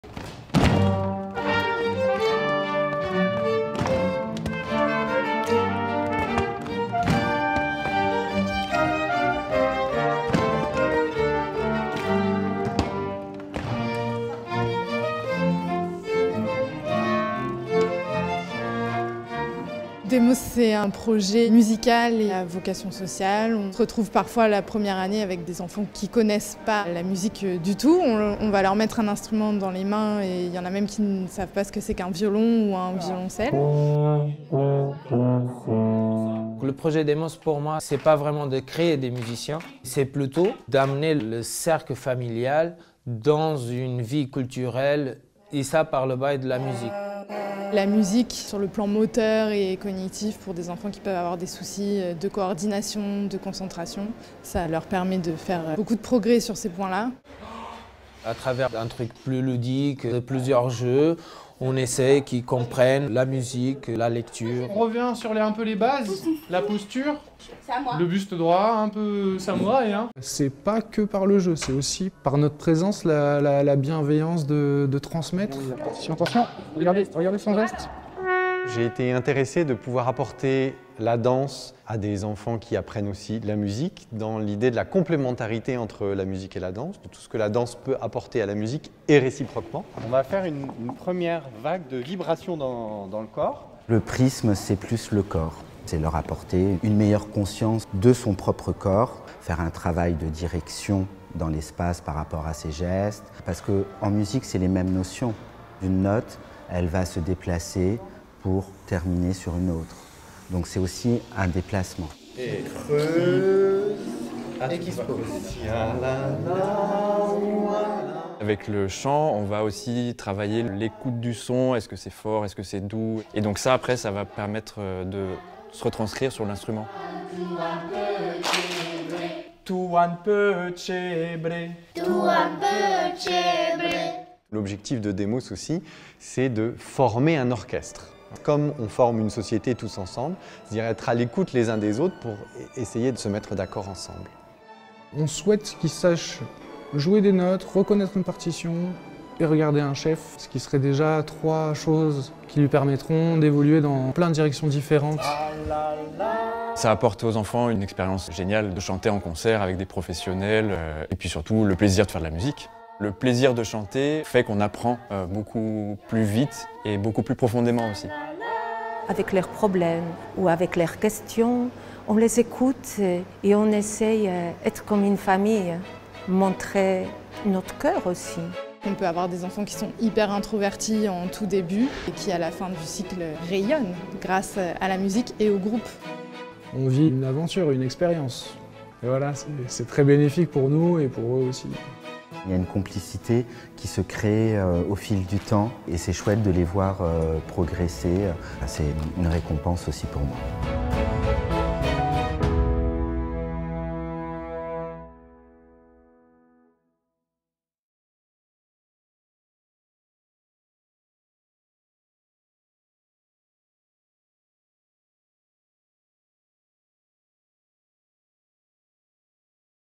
DEMOS : Épisodes #3 - Les intervenants artistiques
Près de 90 enfants vivent leur deuxième année de l’orchestre SQY DEMOS. Suivez-les tout au long d’une web-série dont voici le 3e épisode : les intervenants artistiques